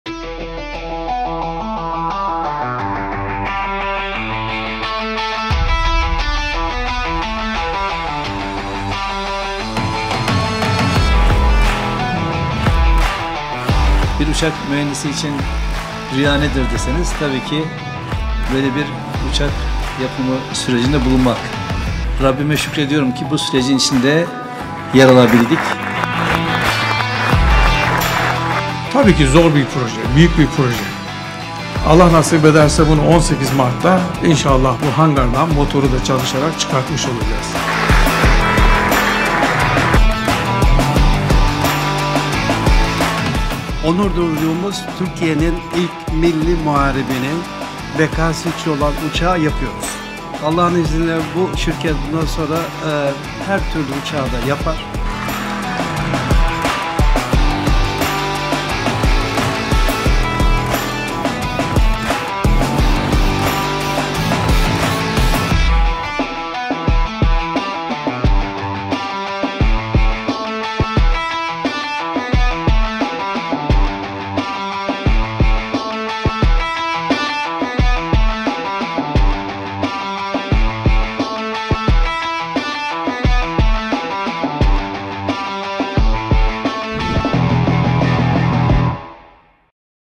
MİLLİ MUHARİP UÇAK SON MONTAJ HATTINA TAŞINDI!
Türk Havacılık ve Uzay Sanayii şirketi olarak 18 Mart 2023'te hangardan çıkarmayı hedeflediğimiz Milli Muharip Uçağımız, son montaj hattına taşındı!